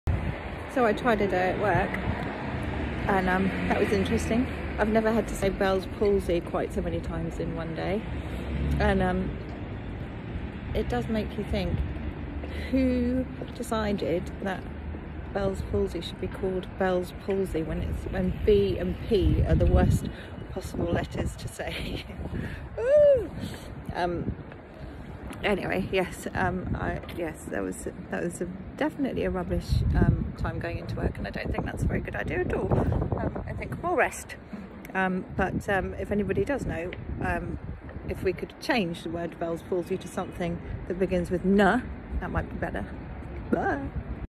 Saying B and P with Bell’s Palsy
The irony of having a facial paralysis condition that begins with B and P 🙄